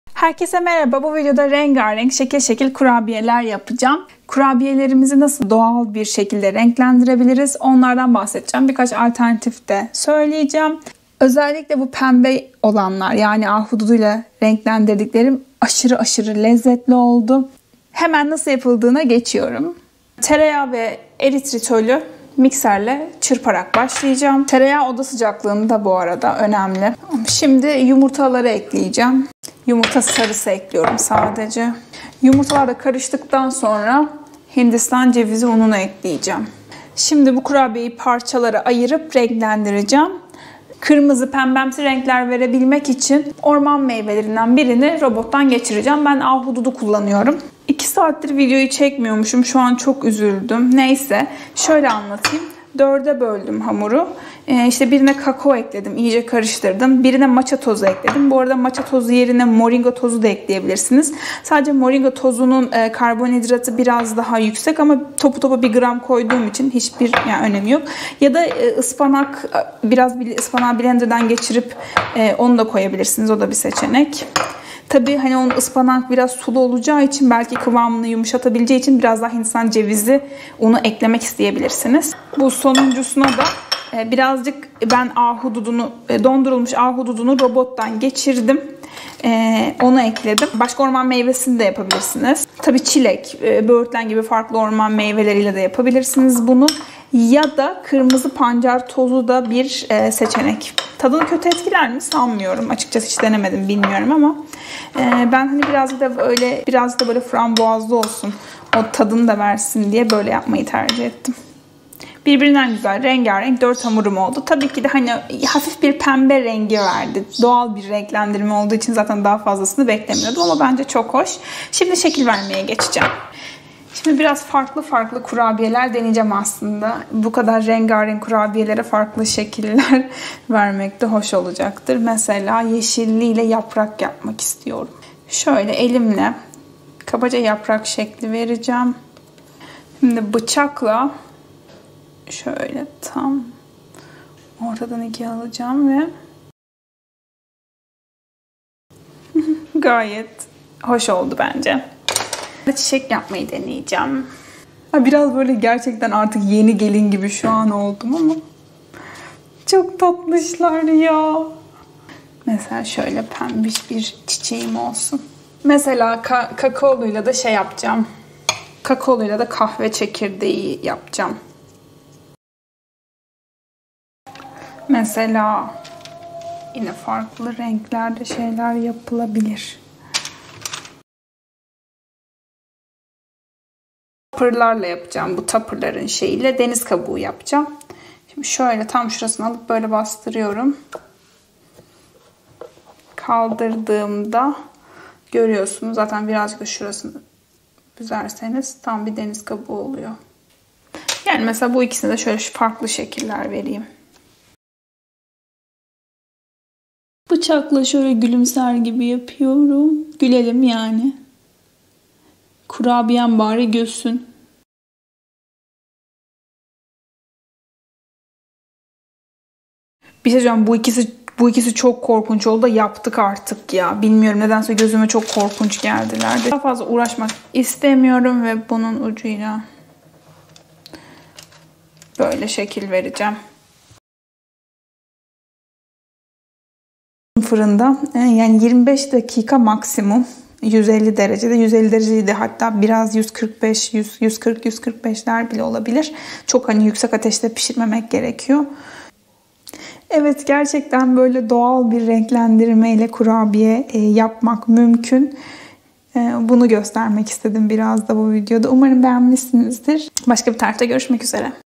Tereyağlı Renkli Kurabiyeler | Ketojenik, Glutensiz, Şekersiz | Doğal Kurabiye Renklendirme
Bu videoda hem tadı hem de görüntüsü çok güzel olan bir kurabiye tarifi veriyorum. Kurabiyeleri doğal bir şekilde nasıl renklendirebileceğinizi de gösteriyorum.

Malzemeler
150 gr tereyağı
42 gr / 3.5 yemek kasigi eritritol
2 yumurta sarısı
56 gr / 1/2 su bardağı hindistan cevizi unu

Renklendirmek için

3 gr /  1  tatlı kaşığı Kakao
1 gr / 1/2 tatlı kaşığı Matcha tozu / Moringa tozu
5-6 adet dondurulmu; ahududu / böğürtlen / çilek / yaban mersini

100 gramındaki makrolar

Yağ  48.28
Net karb  6.38
Protein  3.13
Kalori  493.69